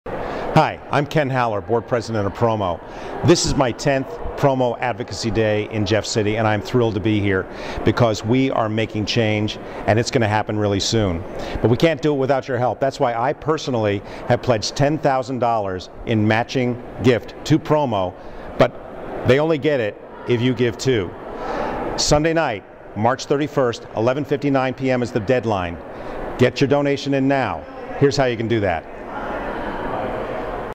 PROMO Board Chair, Ken Haller, Matches All Donations Until March 31!
Ken Haller, PROMO's Board Chair, will match all donations PROMO receives, up to $10,000 until March 31 at 11:59pm!